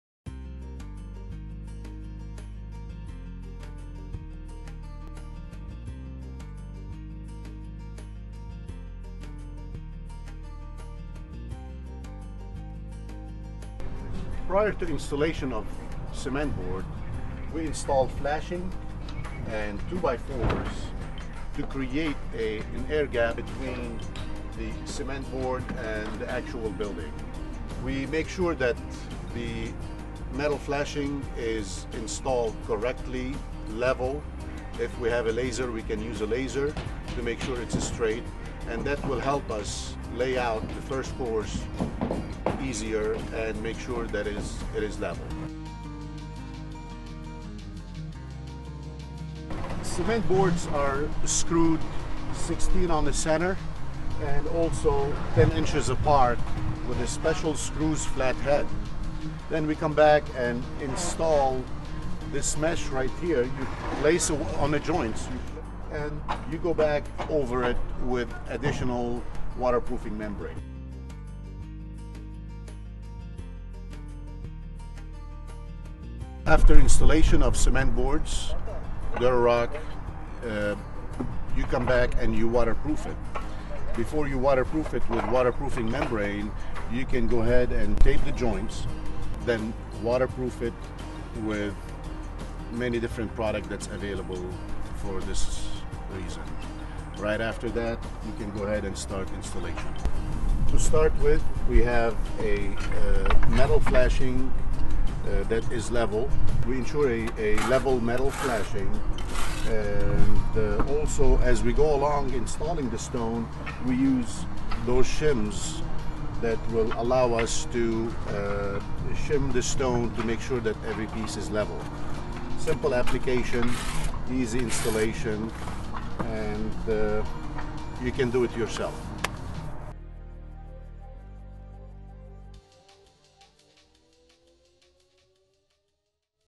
Natural Stone Installation
A quick guide for installing natural stone veneer products on your home or business!